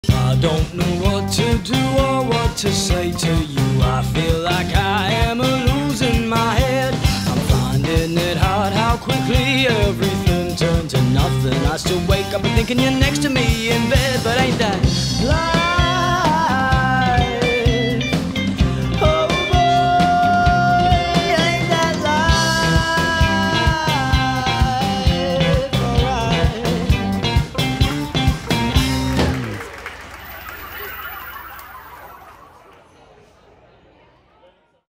Live on the Cargo Stage
A sample of some of the amazing artists who have performed at Miami Marketta's free live music stage.